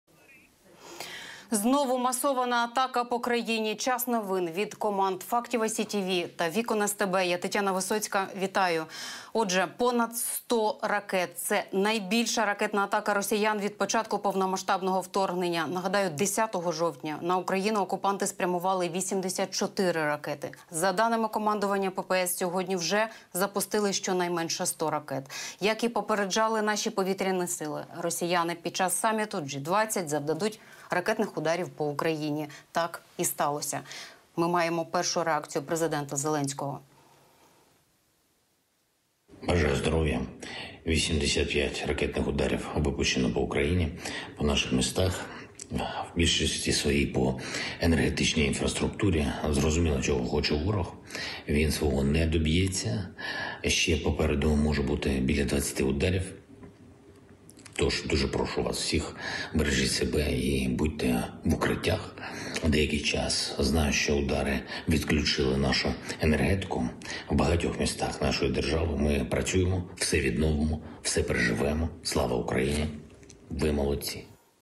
⚡️ТЕРМІНОВО! ЗЕЛЕНСЬКИЙ звернувся до українців
Президент України Володимир Зеленський прокоментував масований ракетний обстріл РФ по Україні.